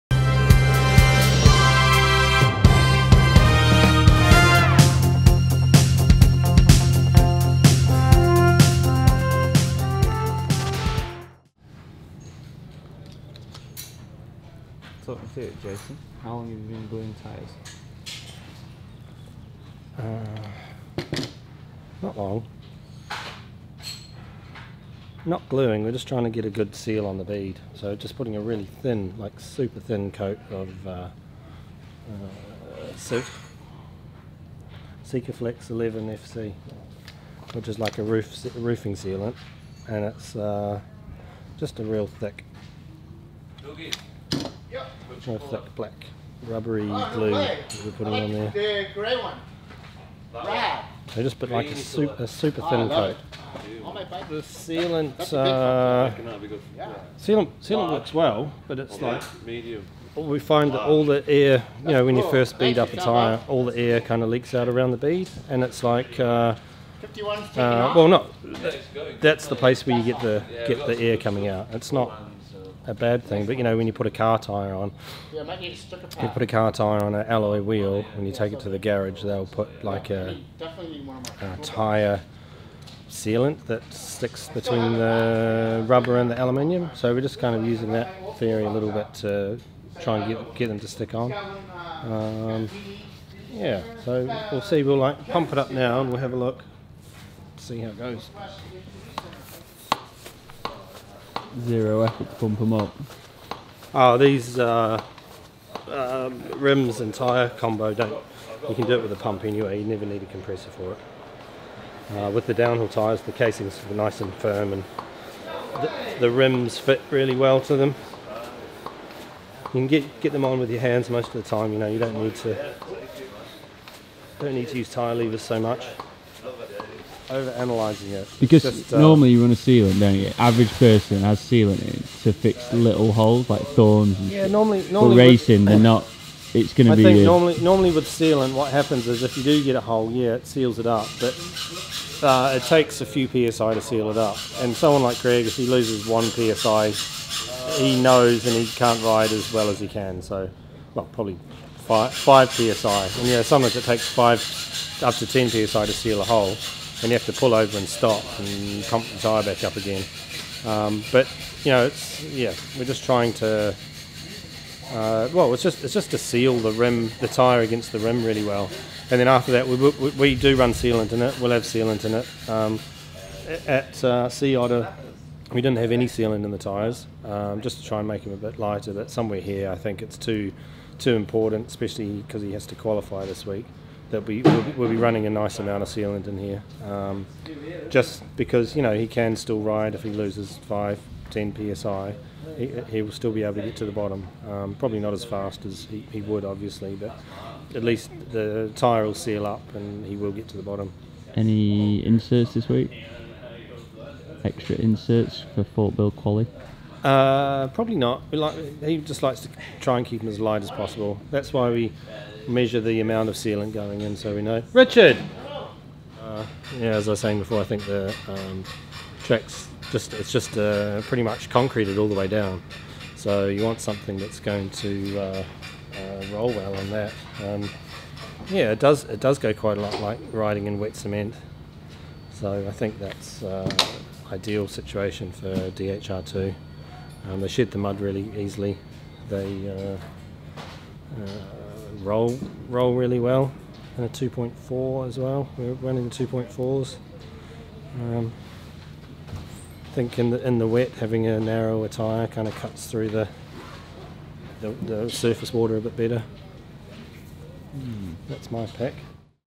Marshy's Fort William Tyre Setup for Greg Minnaar
Roofing glue? What will he do next... Catch up with Jason Marsh as he puzzles his way through a new tyre mounting technique for Greg Minnaar's, Fort William V10 setup.